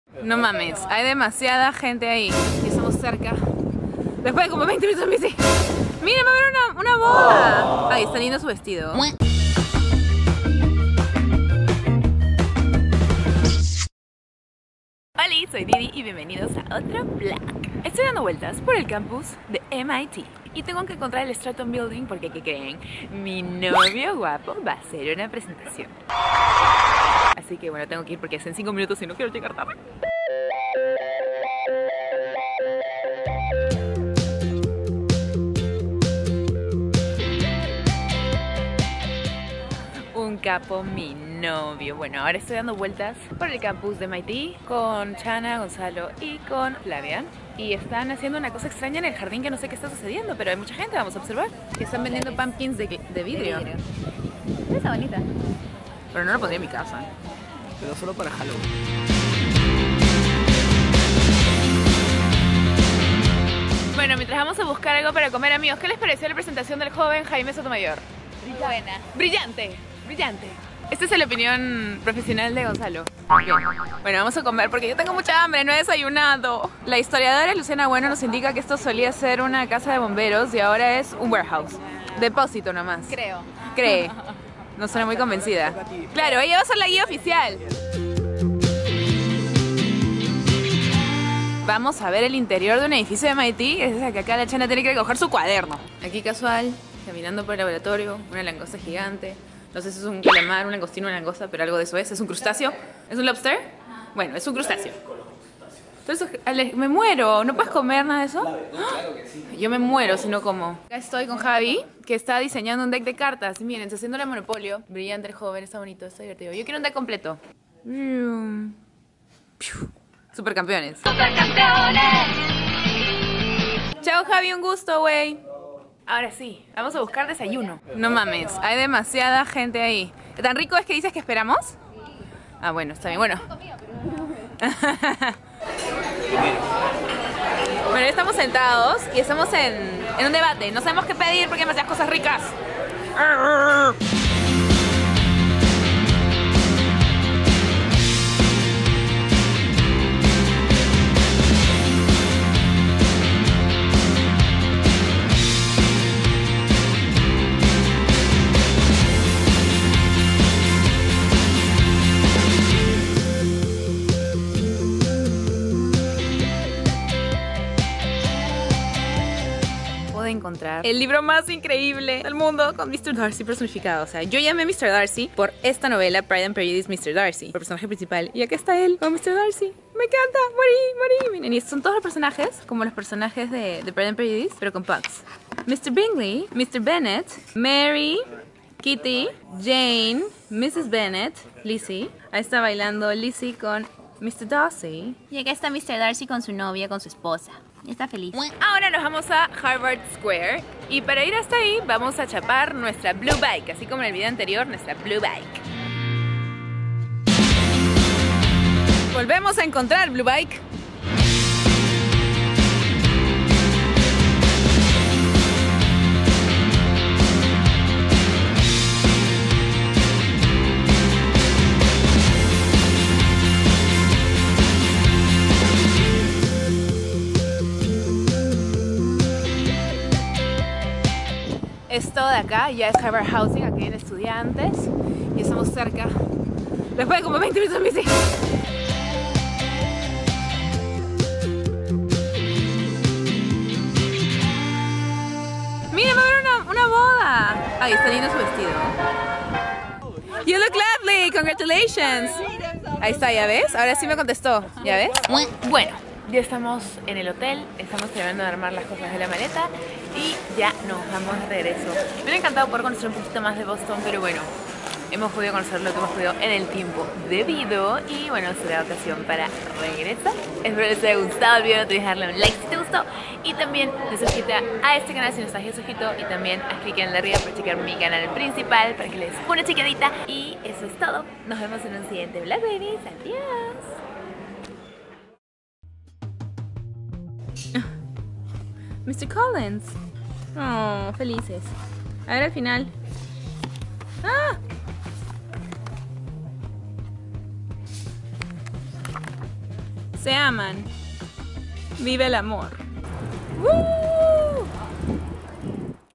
La Forma Más BARATA de Transportarse en BOSTON | Dolce Didi
Damos un recorrido por Boston y les muestro la forma MÁS BARATA de poder transportarse y conocer esta ciudad.